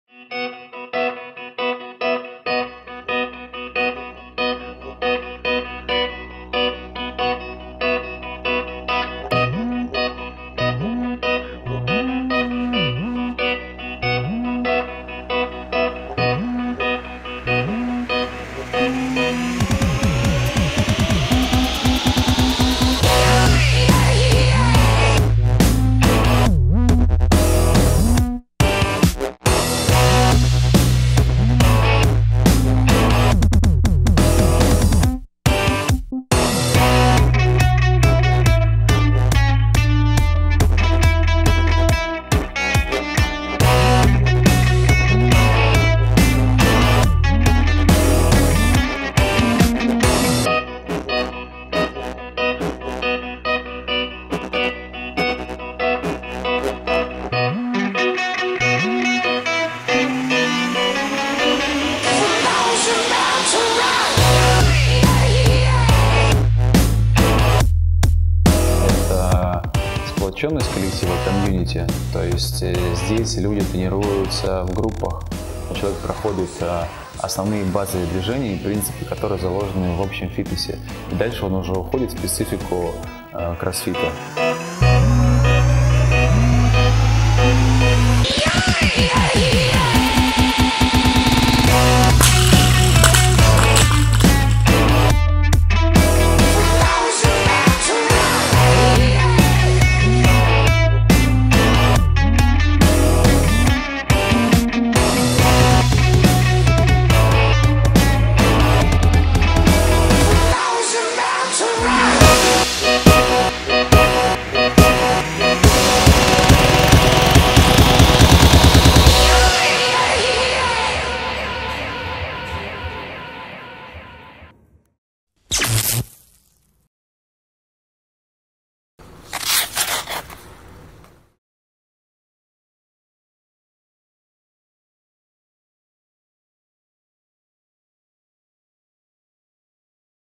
Что такое кроссфит/crossfit
Рассказываем в зале BackStage Crossfit в Петроградском районе Петербурга.